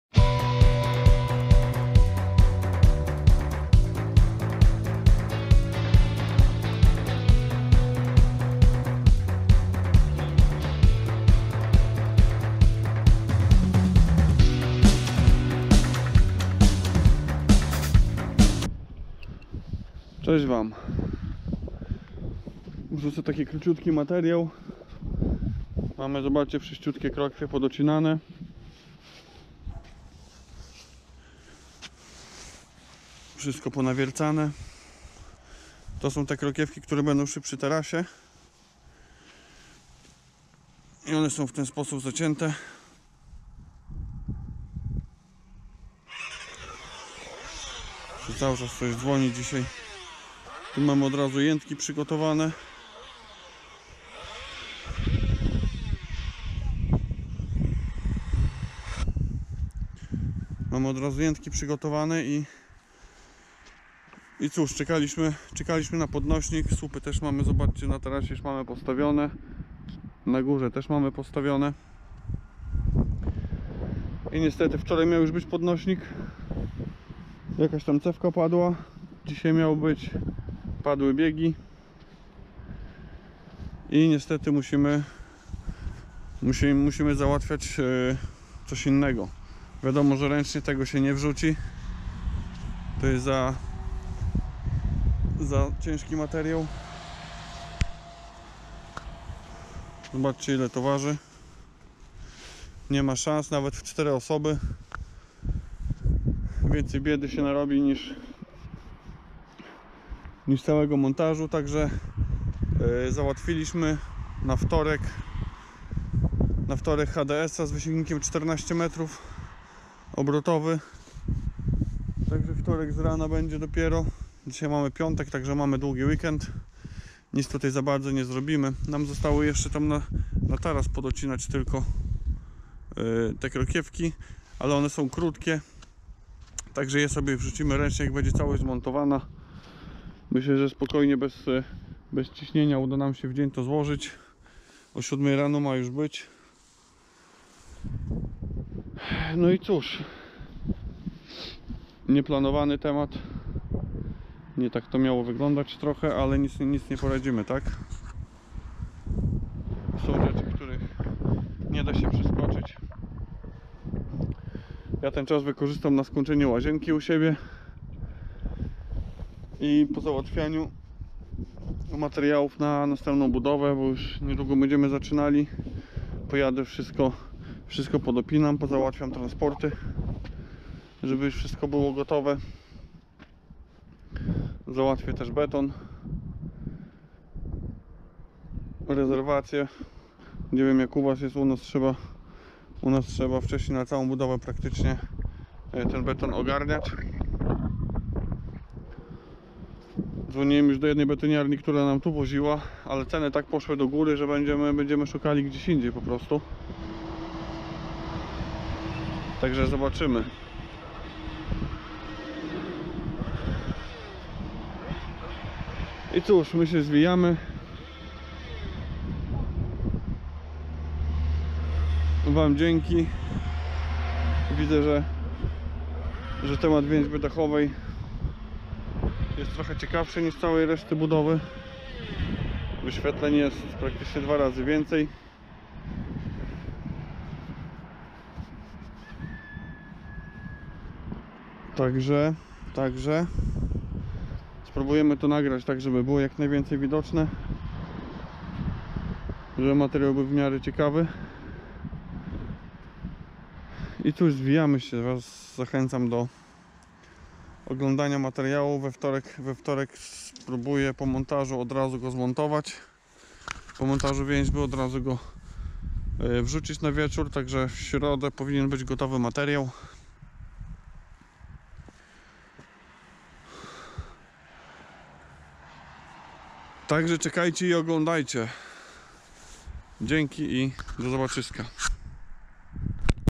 Więźba dachowa - Realia budowy i NIEUDANY MONTAŻ
Więźba dachowa - Realia budowy i NIEUDANY MONTAŻ. W życiu zdarzają się różne sytuacje, zwłaszcza jeśli mowa o placu budowy. Miał być fajny montaż więźby a zobaczcie co wyszło